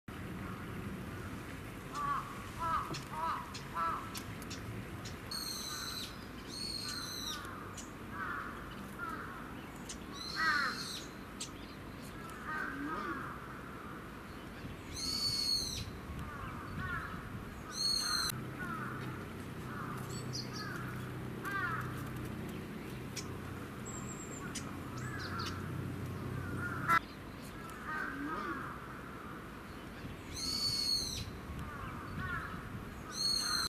[4K] 野鳥：ウグイス／Ｎ区Ｓ公園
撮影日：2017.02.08
藪の中を動き回るウグイスです。
カメラで追うのも大変です。